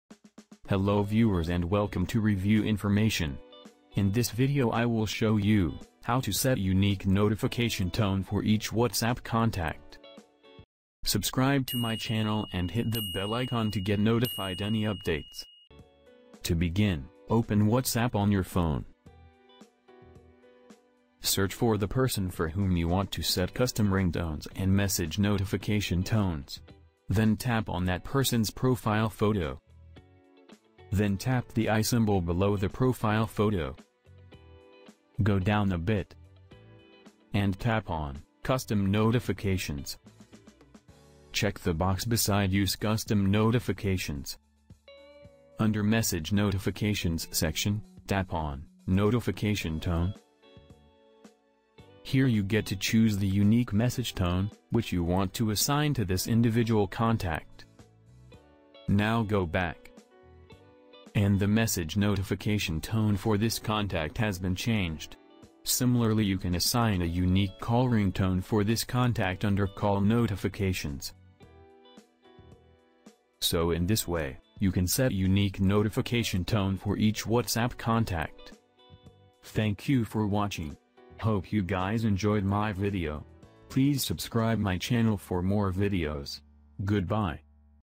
How to Set Different Notification Tone for Each WhatsApp Contact
In this video I will show you, How to Set Different Notification Tone for Each WhatsApp Contact.

Open WhatsApp on your phone and search for the person for whom you want to set custom ringtones and message notification tones. Then tap on that person's profile photo. Tap on the i symbol below the profile photo. Go down a bit, and tap on custom Notifications. Check the box beside Use custom notifications.

Under Message notifications section, tap on Notification tone. Here you get to choose the unique message tone which you want to assign to this individual contact. Now go back. And the message notification tone for this contact has been changed. Similarly you can assign a unique call ringtone for this contact under Call notifications.